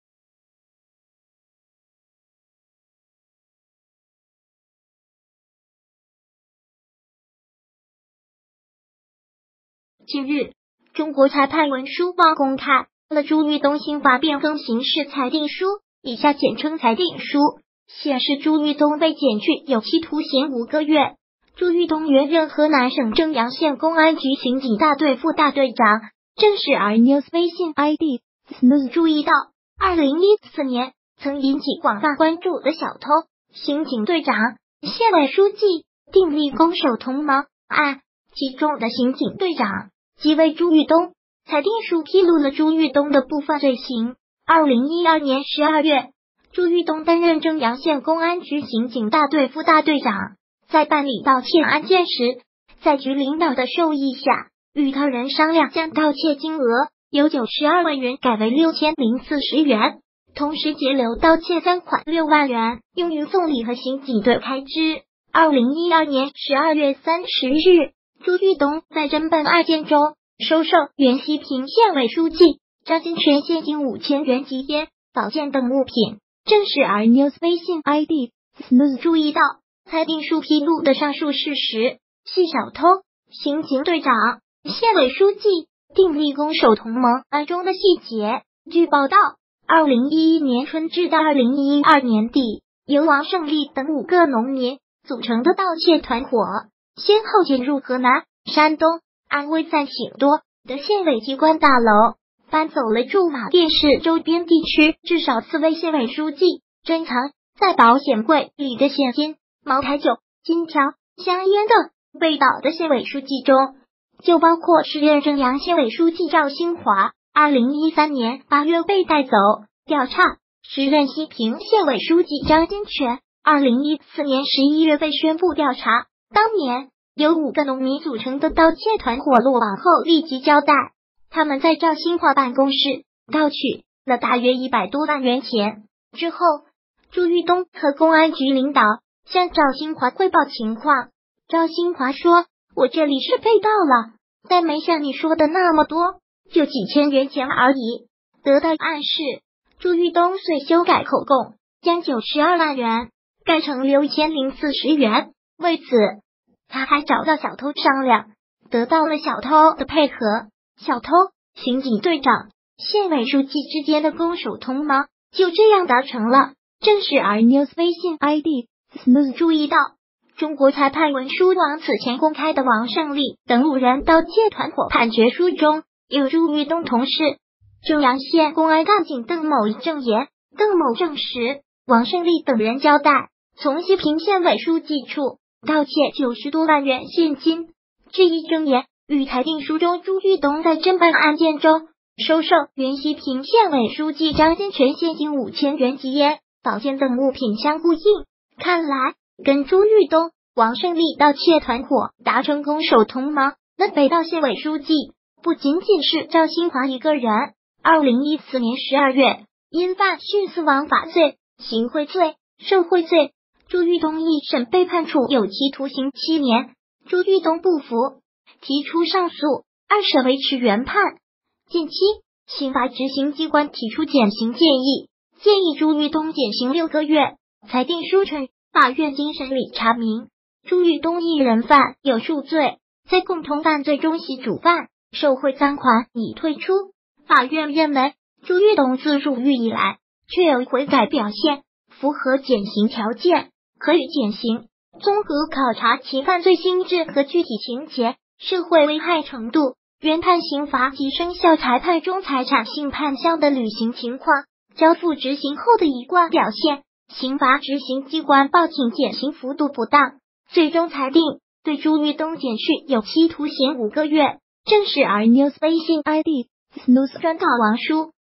跟县委书记、小偷订立“攻守同盟”的刑警队长，减刑了
近日，中国裁判文书网公开了《朱玉东刑罚变更刑事裁定书》（以下简称裁定书），显示朱玉东被减去有期徒刑五个月。朱玉东原任河南省正阳县公安局刑警大队副大队长。“政事儿News”（微信ID：zsenews）注意到，2014年曾引起广泛关注的“小偷、刑警队长、县委书记订立‘攻守同盟’”案，其中的刑警队长，即为朱玉东。裁定书披露了朱玉东的部分罪行。2012年12月，朱玉东担任正阳县公安局刑警大队副大队长，在办理盗窃案件时，在局领导的授意下，与他人商量将盗窃金额由92万元改为6040元，同时截留盗窃赃款6万元用于送礼和刑警队开支。2012年12月30日，朱玉东在侦办案件中，收受原西平县委书记张金泉现金5000元及烟、宝剑等物品。“政事儿News”（微信ID：zsenews）注意到，裁定书披露的上述事实，系“小偷、刑警队长、县委书记订立‘攻守同盟’”案中的细节。据报道，2011年春至到2012年底，由王胜利等五个农民组成的盗窃团伙，先后潜入河南、山东、安徽三省多地县委机关大楼，搬走了驻马店市周边地区至少4位县委书记珍藏在保险柜里的现金、茅台酒、金条、香烟等。被盗的县委书记中，就包括时任正阳县委书记赵兴